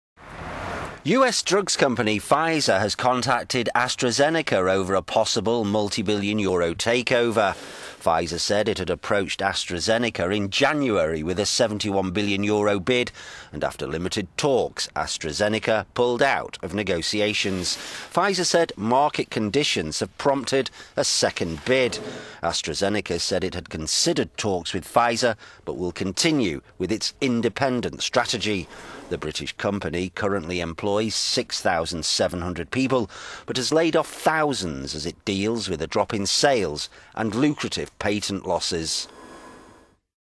Drugs giant Pfizer's move on AstraZeneca rebuffed - economy
US drugs company Pfizer has contacted AstraZeneca over a possible multi-billion euro takeover.

Pfizer said it had approached AstraZeneca in January with a 71 billion euro bid and after limited talks AstraZeneca pulled out of negotiations.

Pfizer said market conditions have prompted a second bid.

AstraZeneca said it had considered talks with Pfizer, but will continue with its independent strategy.

The British company currently employs 6,700 people, but has laid off thousands as it deals with...